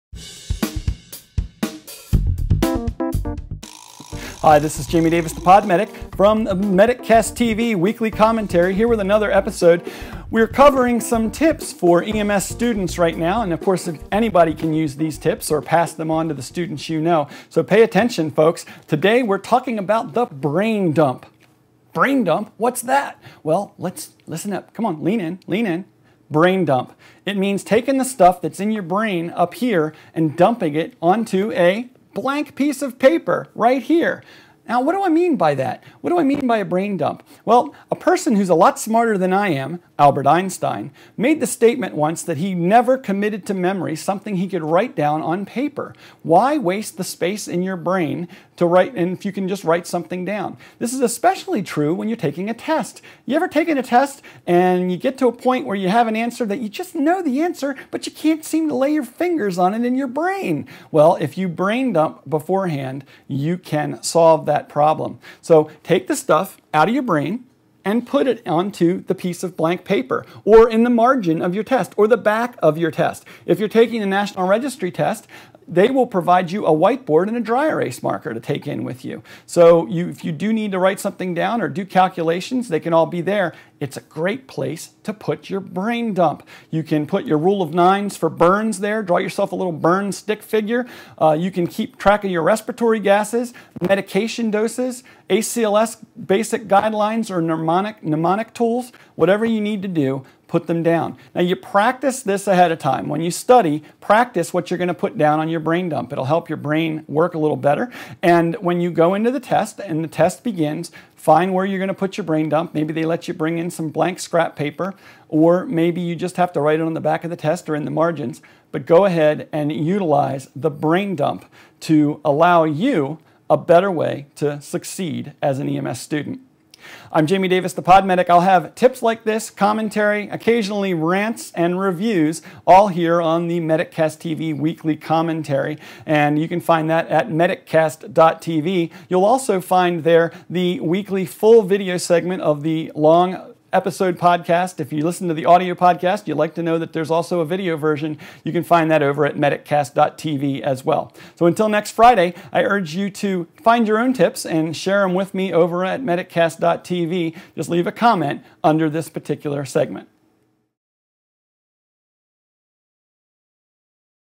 EMS Test Magic Brain Dump - MedicCast TV Weekly
MedicCast TV Weekly Video Commentary
&mdash;-
Since  so many new students are starting their EMT and Paramedic classes right  now, I thought I would put out another student study and testing tip  for this week's show here on the MedicCast TV EMS podcast.
Magic EMS Testing Brain Dump
Albert Einstein once said that he never memorizes something he  can write down on paper. He understood the magic of the "Brain Dump." He  knew that when you put something down on paper, you free up your mind  to remember other things.
When you are testing in your paramedic or EMT classes, you can use the "Brain Dump."
When you "Brain Dump" you wait until the test begins then "dump"  your memorized material onto your scratch paper, the back of your test,  or even the margins of your test. This is perfectly alright. You've not  brought anything in to the test but what you carried in your mind.  Because you've "dumped" these memories on the paramedic, EMT, or anatomy  test, your mind is now free to review other material and concepts  during the remainder of the test.
What's a Paramedic Brain Dump?
So what does the paramedic student "dump" onto his page before a  test? My recommendation is to choose things that are easy to quickly  write down like a "rule of 9's" burn chart with a stick figure, or a  list of numbers to help you remember values for respiratory gases. You  can also note commonly used calculations and formulas.
When you  are studying, practice your "brain dump" so you know what you will write  during your EMS test. Keep it simple and limit your "brain dump" to  just a few items that you really need to keep straight. Good luck - You Can Do It!!!
&mdash;&mdash;&mdash;-